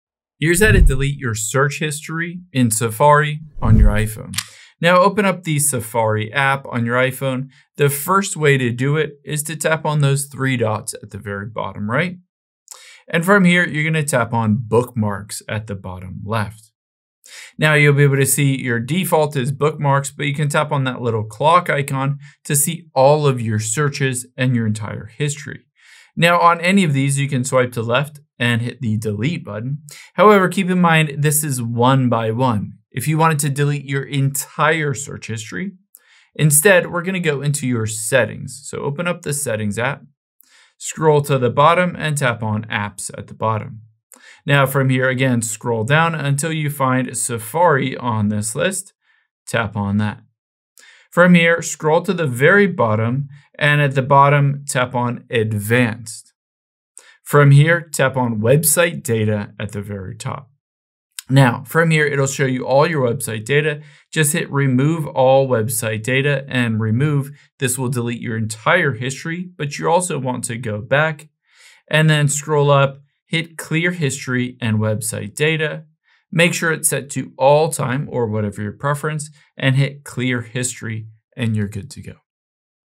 How To Delete Search History In Safari On iPhone
Here's how to delete your search history in the Safari web browser on your iPhone.